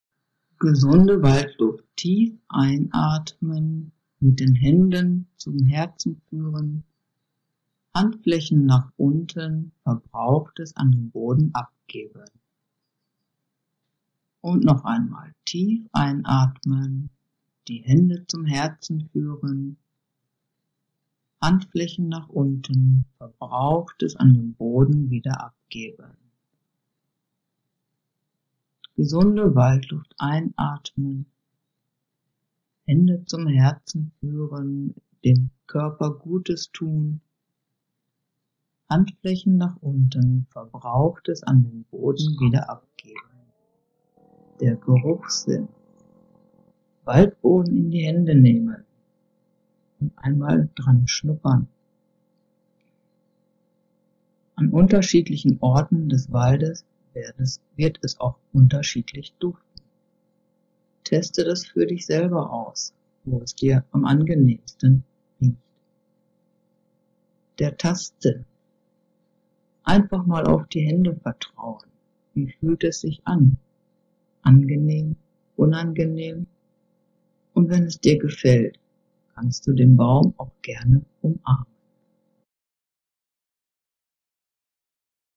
Waldbaden Übungen
Waldbaden - Natur-Erfahrung mit allen Sinnen. Hier mal kurze Eindrücke verschiedener Übungen. Naturerfahrung, mit allen Sinnen, Gesundheit aus dem Wald